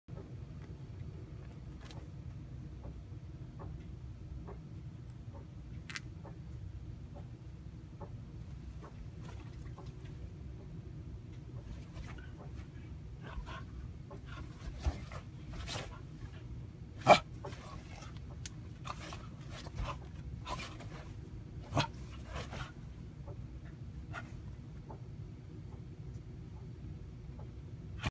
Corgi plays with bird
He thinks it's play, she thinks it's war.